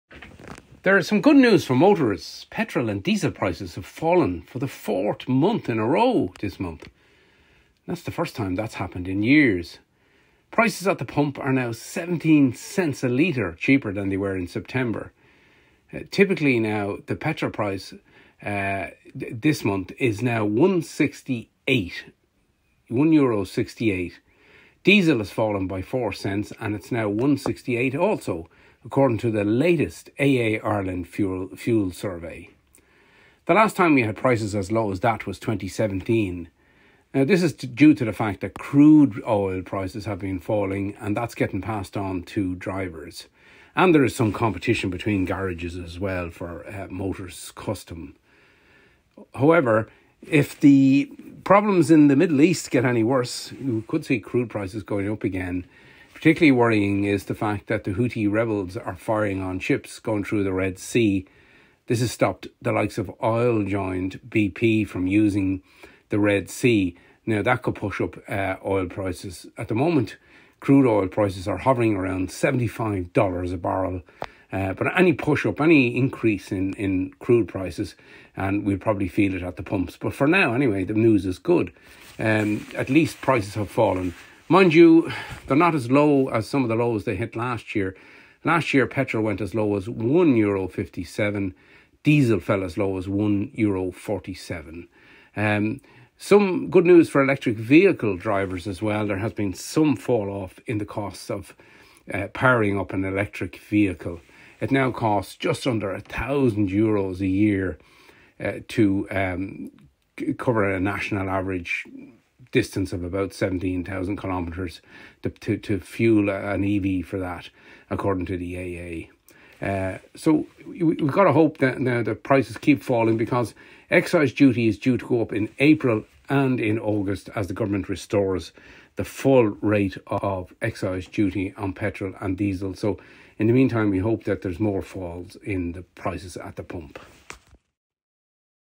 Petrol and diesel prices fall for the fourth month in a row
Petrol and diesel prices fell for the fourth month in a row this month, the first time this has happened in years. Petrol is down from €1.72 per litre in December to €1.68. Diesel also fell by 4c per litre, from €1.72 to €1.68, according to the latest AA Ireland fuel survey.

Prices are now 17c cheaper than they were in September. The last time both prices fell four months in a row was in 2017. However, there are warnings that the conflict in the Middle East, particularly attacks on ships in the Red Sea, could send pump prices up again.